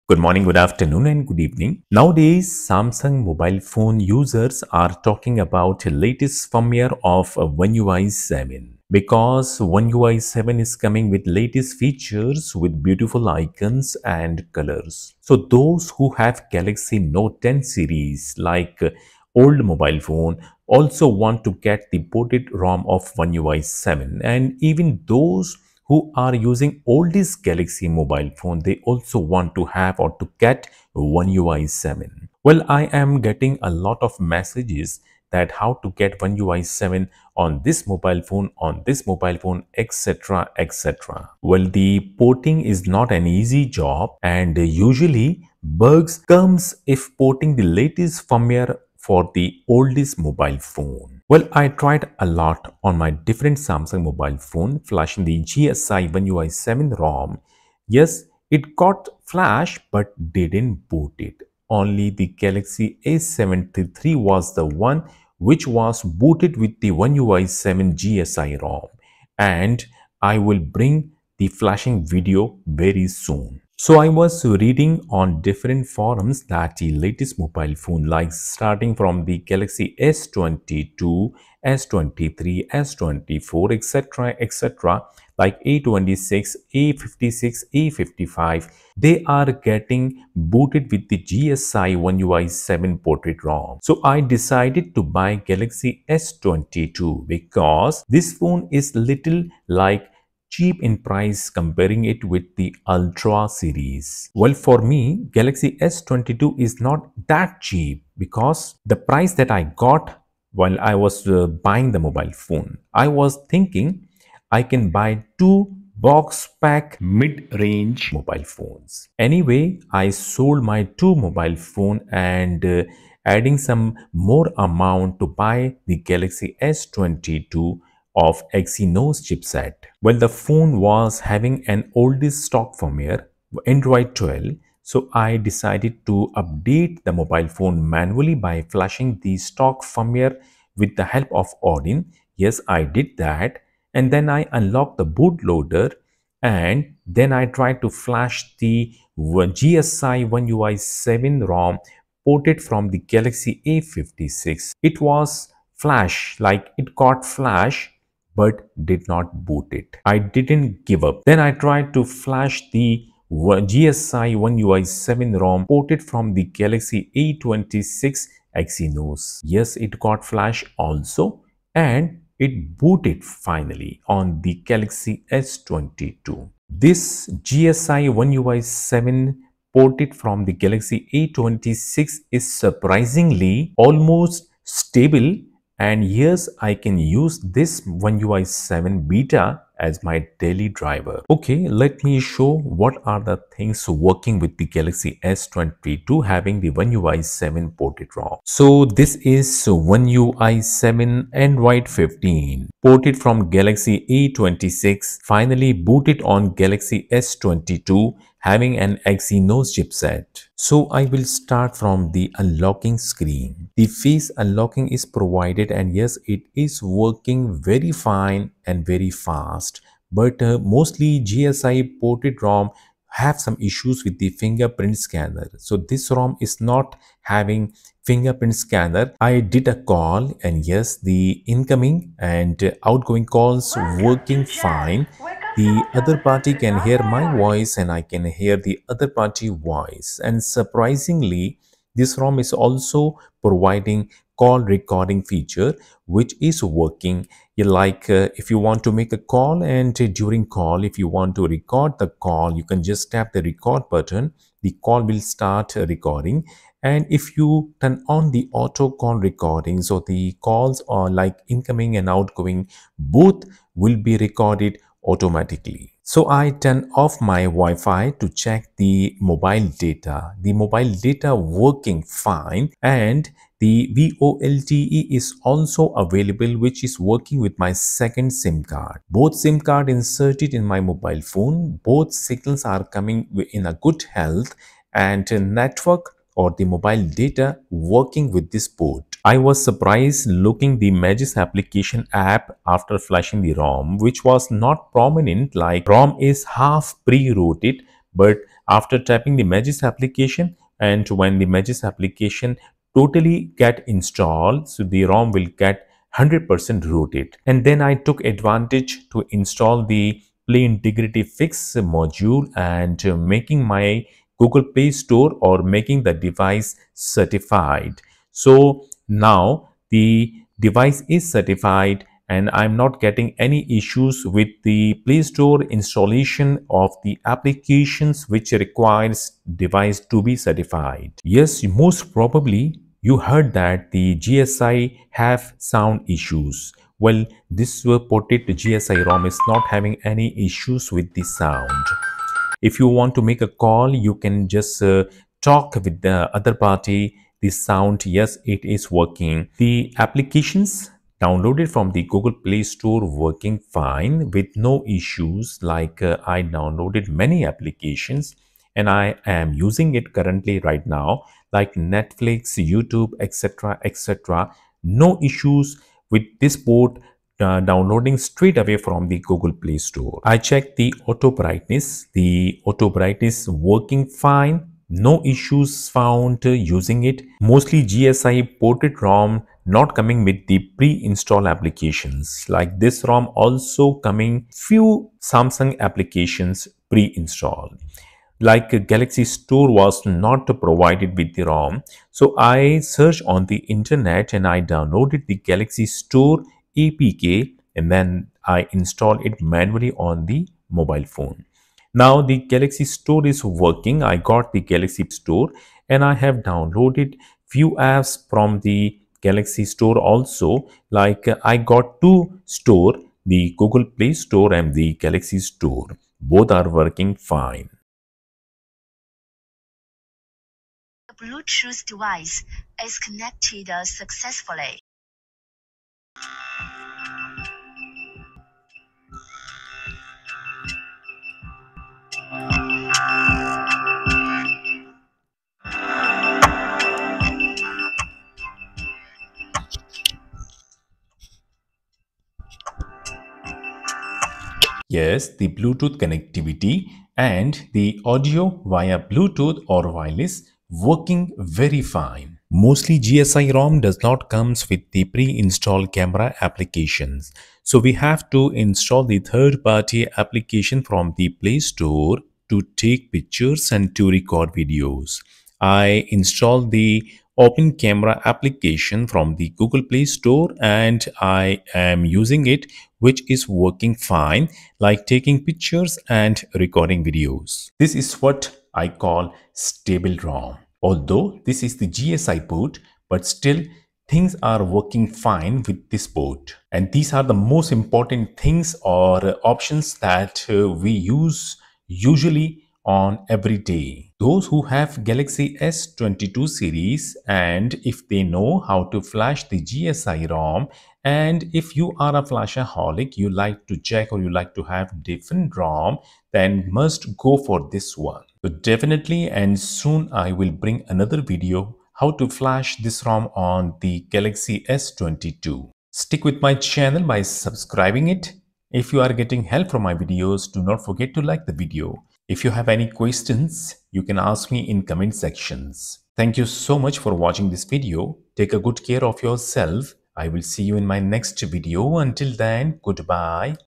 I Got Stable ONE UI 7 GSI Android 15 ON Galaxy S22 -English-
OneUI 7.0
Ported from SM-A266B (a26x)
Ported From Galaxy A26
Exynos 1380 (5 nm) - Global,Exynos 1280 (5 nm) - LATAM
Info:
Device brand: samsung
Device manufacturer: samsung
Device model: SM-A266B
Device codename: a26x
Device board: erd8835
Android version: 15
Android API: 35
Build fingerprint: samsung/a26xxx/a26x:13/TP1A.220624.014/A266BXXU1AYB5:user/release-keys
Build type: a26xxx-user
Build tags: release-keys
Build ID: AP3A.240905.015.A2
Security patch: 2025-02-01
Raw Image Size: 6.39 GiB

Must Unlock Bootloader first

Stay Blessed